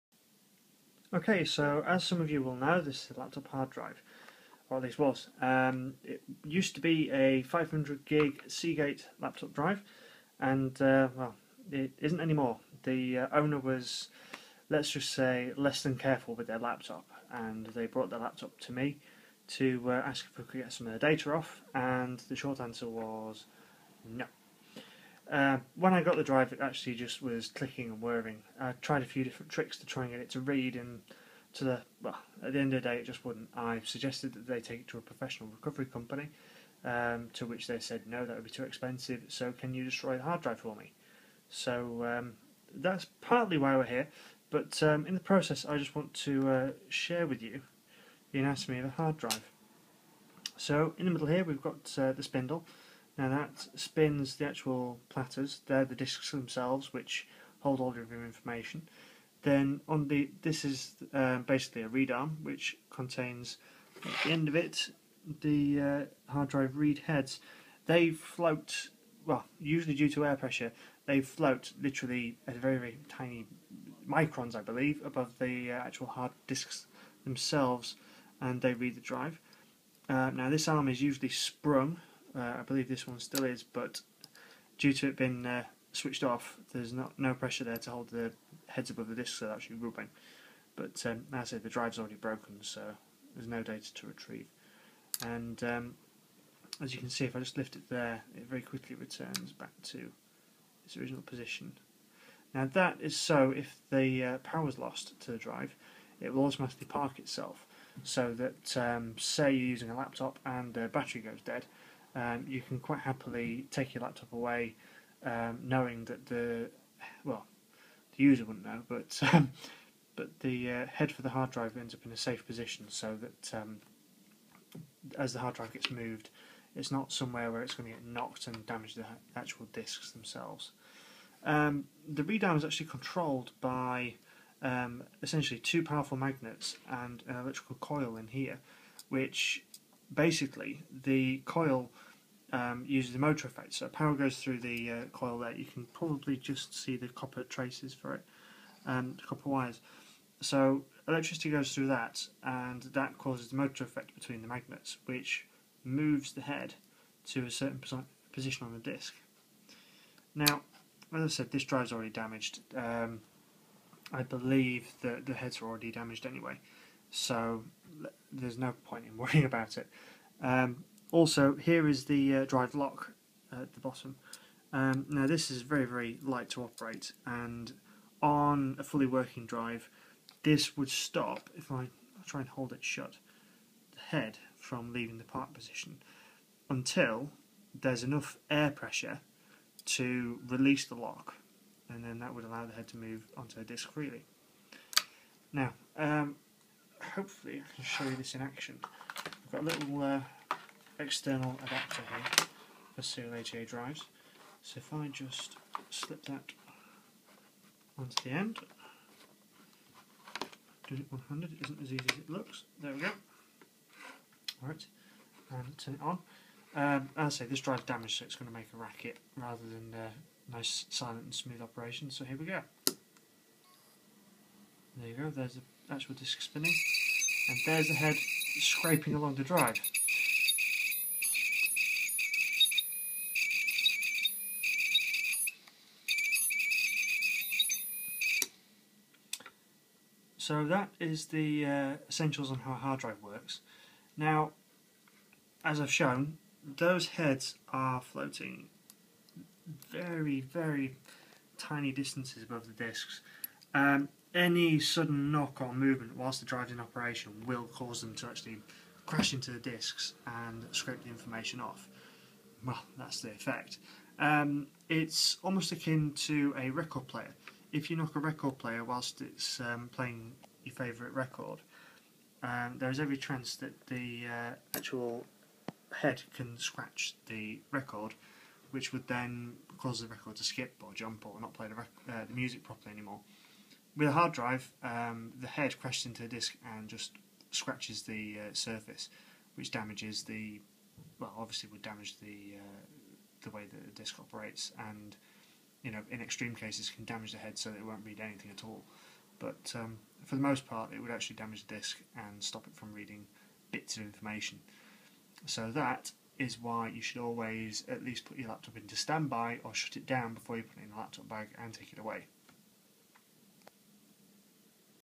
Hard drive dissection - Part 1
In this video I take the top off a 500GB Seagate 2.5" SATA hard drive. I also explain why you should always turn off or standby your laptop before putting it into a bag or moving it around.

The hard drive was already damaged before I did this and the only way to recover any data would have been a professional recovery company, which was beyond what owner was prepared to pay.